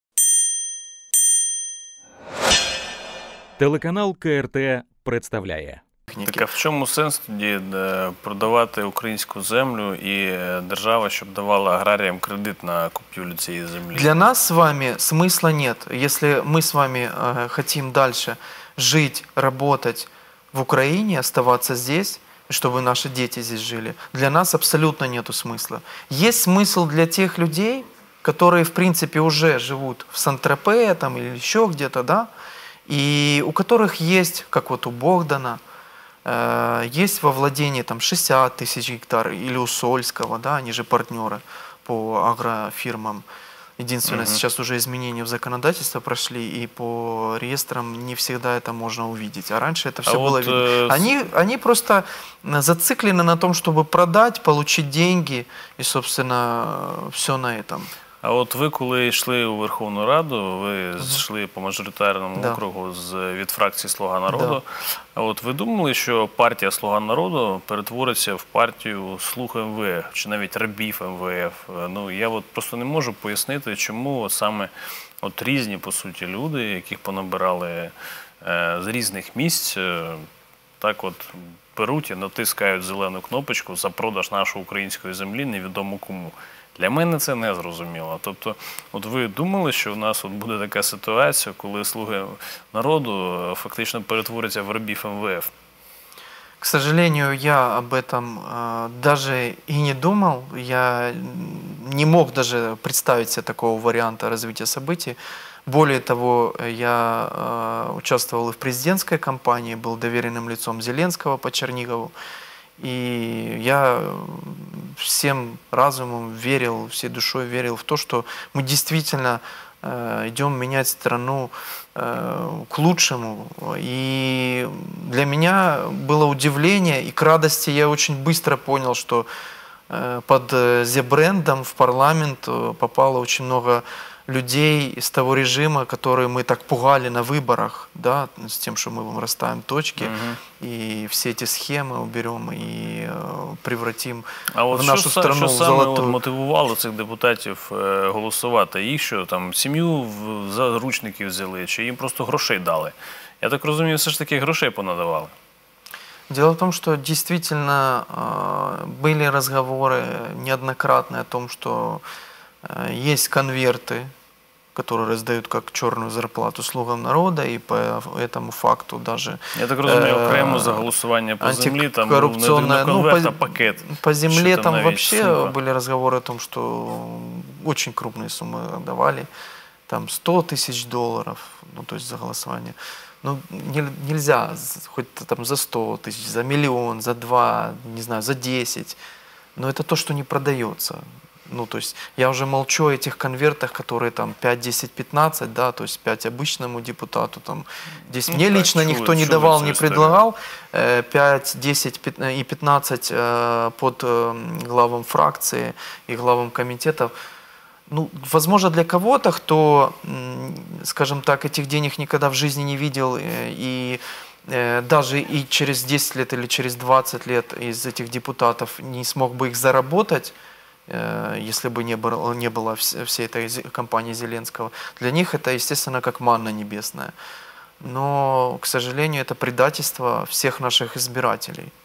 За сколько конвертов депутаты продали землю – нардеп Антон Поляков // Без протокола
Народный депутат Антон Поляков прошел в Верховную Раду по спискам Зеленского, но считает предательством то, в какую сторону пошел президент и его партия. За сколько продались "Слуги народа" и почему нельзя продавать землю, смотрите в видео.

Распространи это видео!